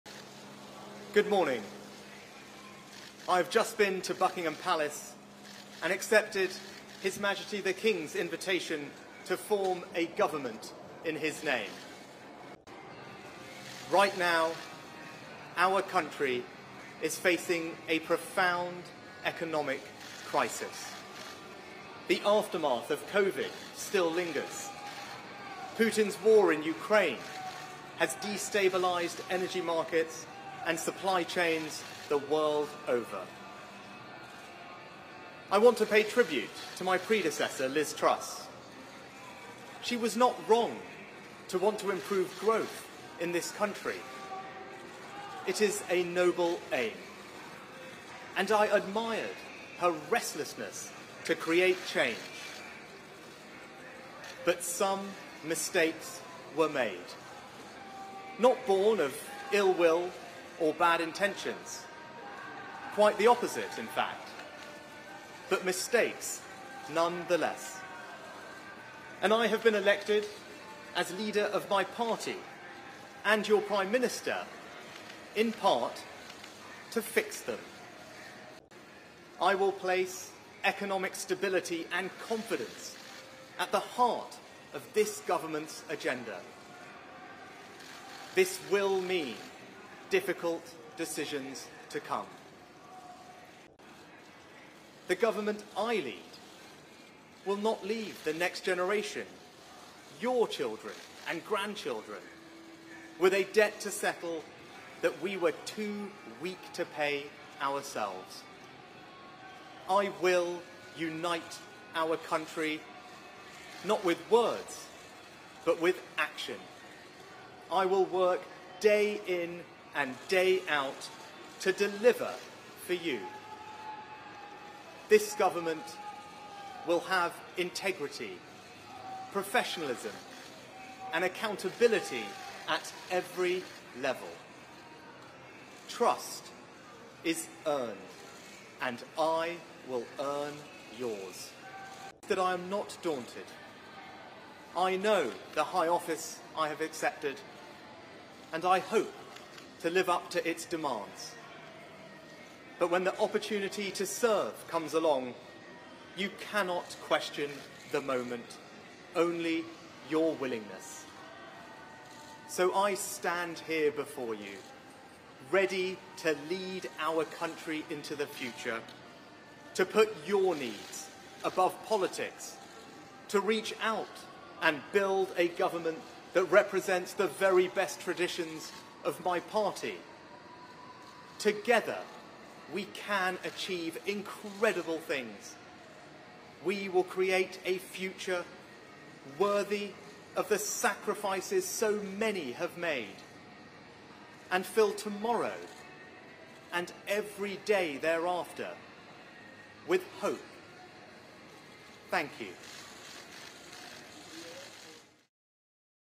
Rishi Sunak pledges to clean up mess left by Truss as UK PM
New British PM Rishi Sunak says he will try to fix the mess left by his predecessor, restore faith in politics and tackle a “profound economic crisis” but warns the country there will be difficult decisions.